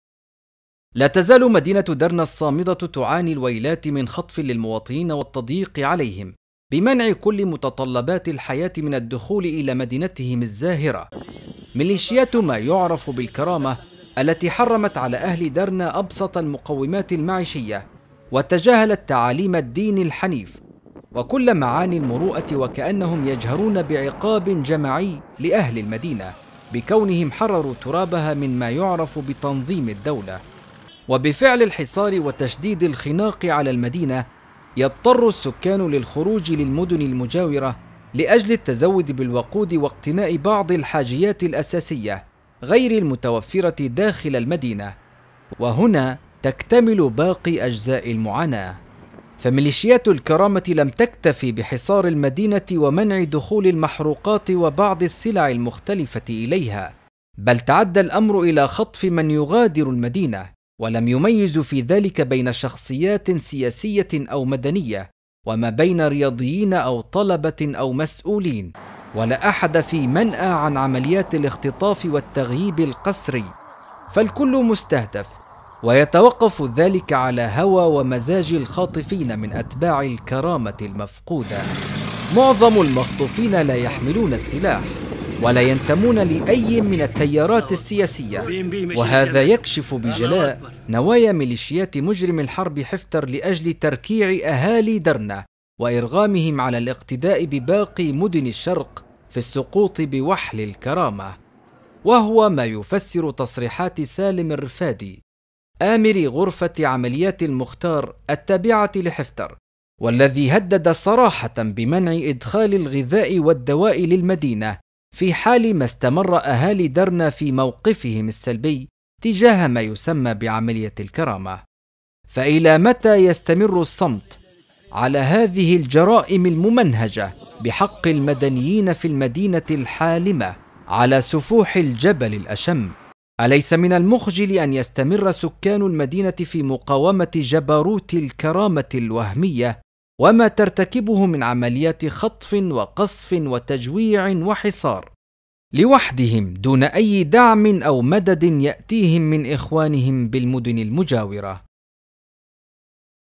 تقرير | الاختطاف بعد الحصار .. ودرنة تتمسك بالانتصار | 11 - 5 - 2017
ــــــــــــــــــــــــــــــــــــــــــــــــــــــــــــــــــــــــــ
ــــــــــــــــــــــــــــــــــــــــــــــــــــــــــــــــــــــــــ
يمكنكم تحميل تطبيقنا TANASUH TV على الهواتف الذكية: